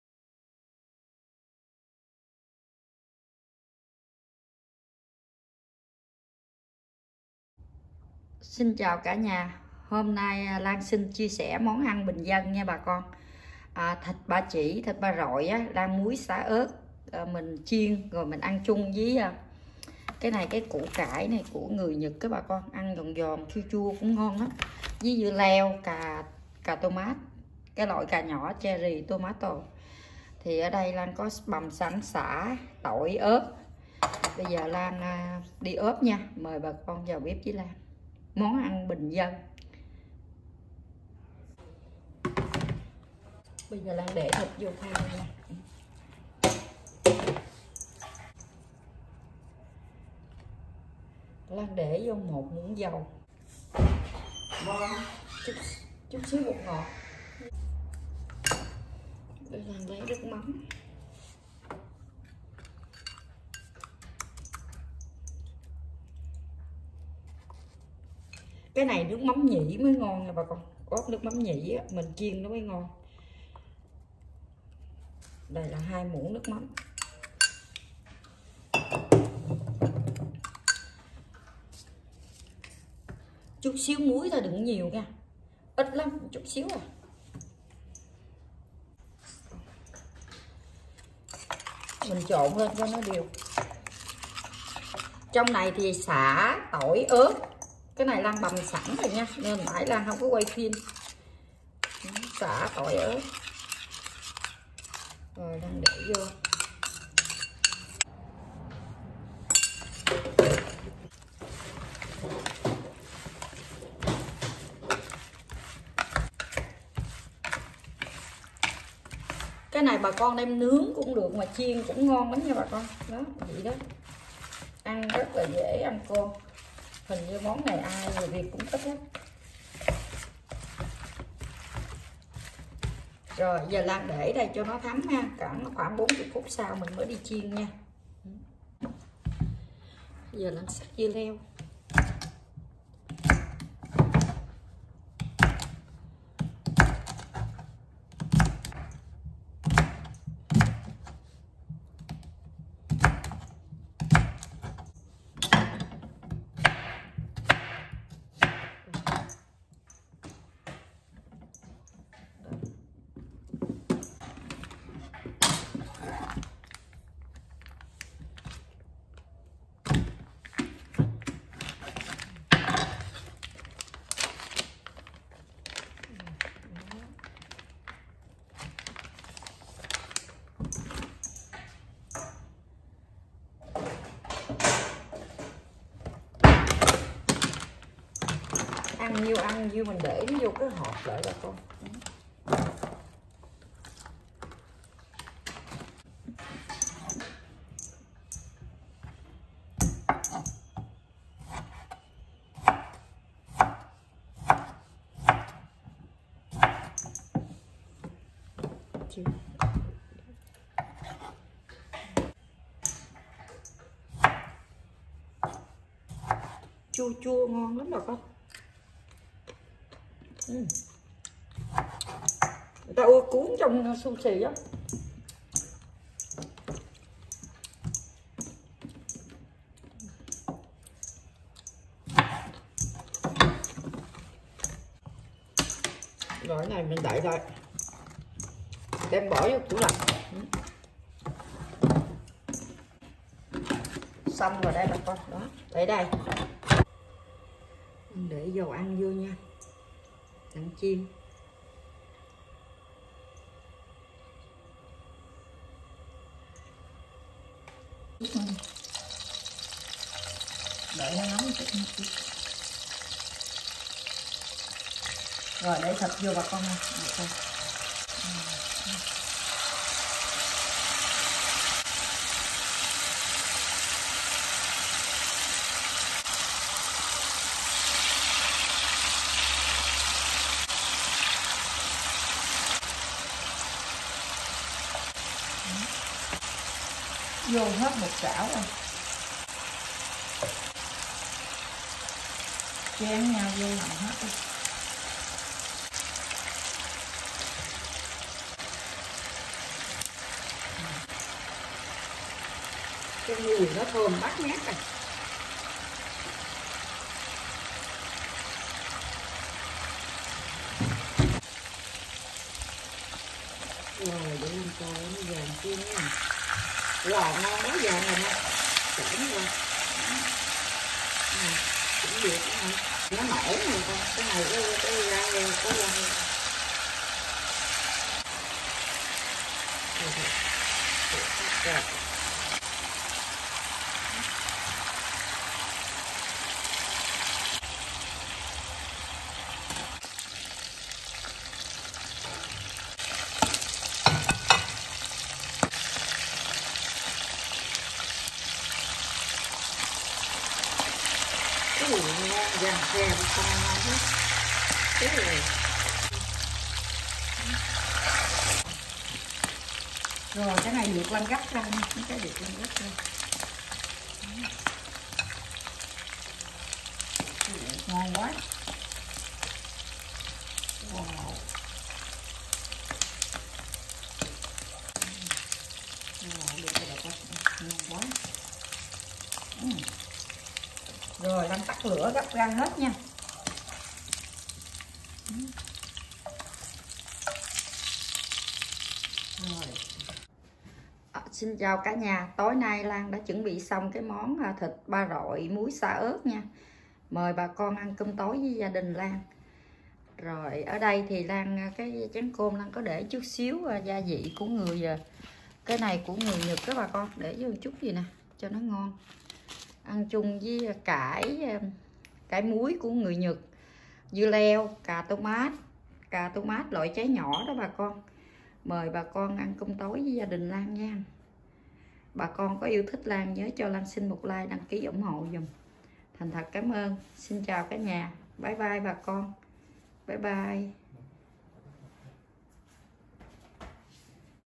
Cuộc Sống Ở Úc | Tập 707 | Món Ngon - Thịt Ba Chỉ Muối Sả Ớt | Salted Pork With Lemongrass And Chili
Xin chào cả nhà hôm nay Lan xin chia sẻ món ngon quê nhà. Thịt ba chỉ muối sả ớt . Ăn với củ cải muối chua Của Nhật. Dưa leo cà chua .Yummy 😋 Cảm ơn tất cả các bạn đã xem video của Lan. Thank you very much❤️❤️❤️❤️